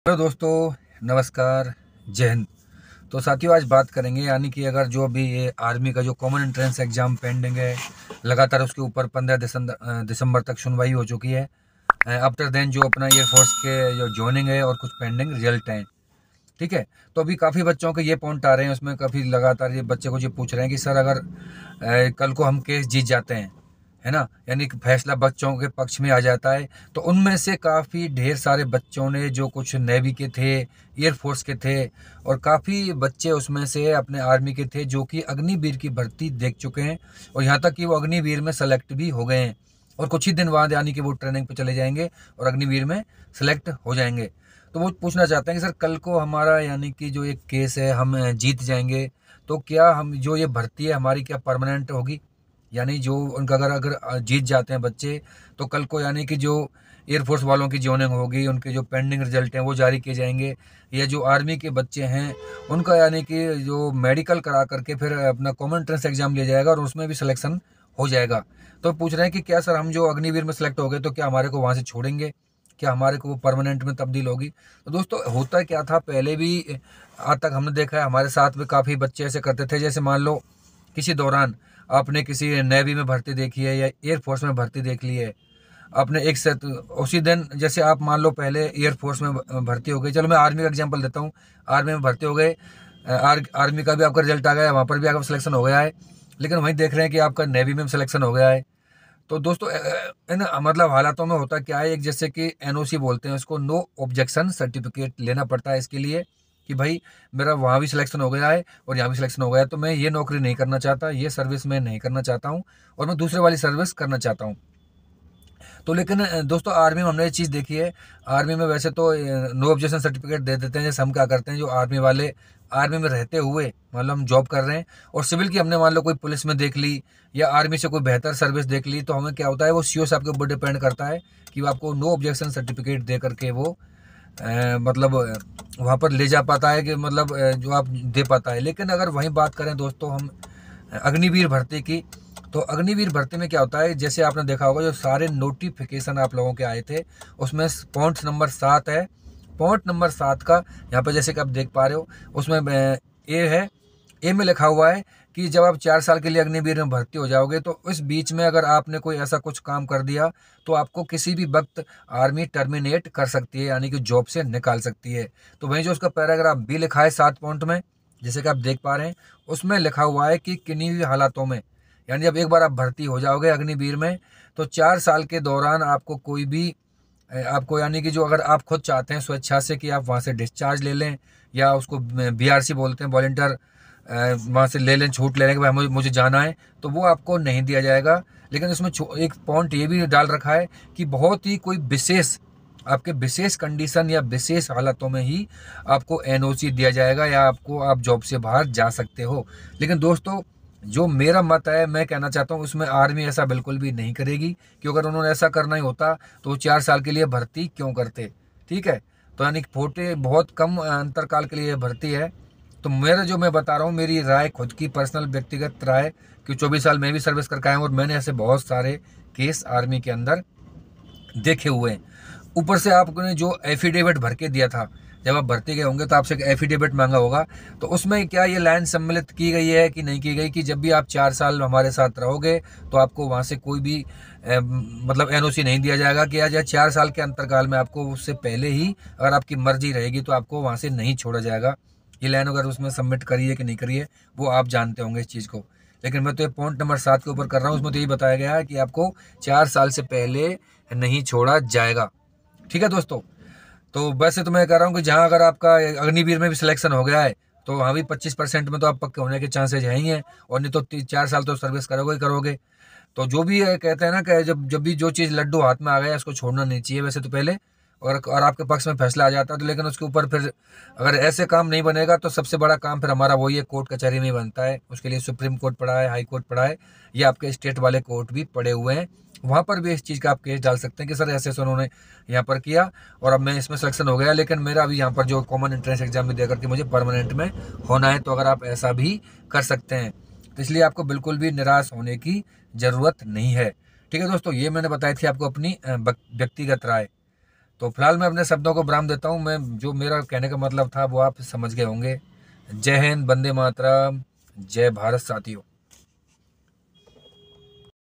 Army Pending Exam Big Updates | जिनका CEE पेंडिंग था और अब वो अग्निवीर में भर्ती हो गए तो क्या होगा
साथियों ये वीडियो उनके लिए है
 जिन बच्चों का Army CEE , Airforce joining and pending Result aana baki thai.
to ab inmein se bahut sare bachche.
Army airforce aur navy mein bharti ho gaye hai.
to agar ye case jeet jate hai.
to kya .inhe Agniveer se NOC deke kya permanent bharti wala joining, CEE dene diya jayega.